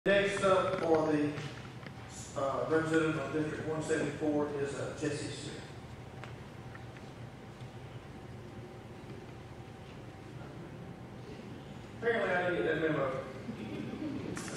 Franklin Bailey - Candidate Introduction @ Candidate Forum - 2014
Valdosta Lowndes County Chamber of Commerce Candidate Forum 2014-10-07